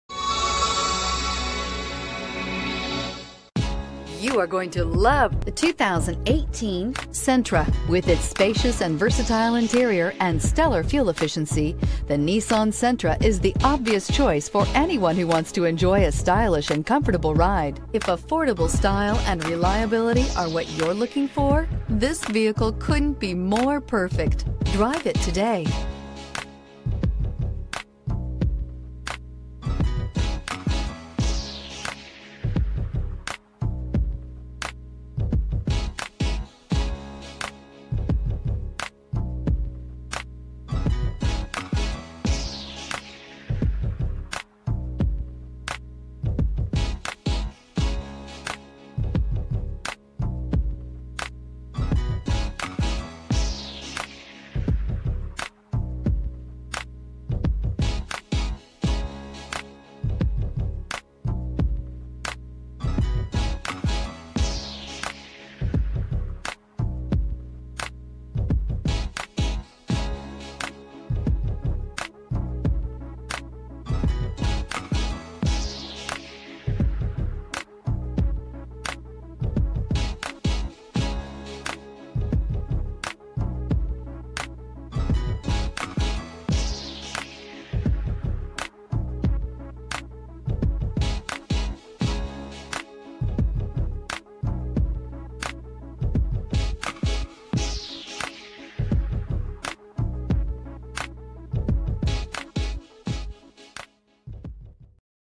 2018 Nissan Sentra Live JY289981
2018 Nissan Sentra S

Alexander Chevrolet Buick GMC
2211 Highway 46 South

bEquipment/bbrProtect this unit from unwanted accidents with a cutting edge backup camera system. Front wheel drive on this small car gives you better traction and better fuel economy. This 2018 Nissan Sentra  gleams with an elegant silver clear coated finish. This model has a 1.8 liter 4 Cylinder Engine high output engine. This unit features cruise control for long trips. Enjoy the tried and true gasoline engine in this vehicle. It has an adjustable telescoping wheel that allows you to achieve a perfect fit for your driving comfort. Anti-lock brakes are standard on this small car. The shiftable automatic in this 2018 Nissan Sentra  gives you to option to control your gears manually without having to give up the convenience of an automatic transmission. Small and nimble this model scoots through traffic.  It offers great fuel mileage and ease of parking.brbrbPackages/bbr[C03] 50 STATE EMISSIONS. Equipment listed is based on original vehicle build. Please confirm the accuracy of the included equipment by calling the dealer prior to purchase.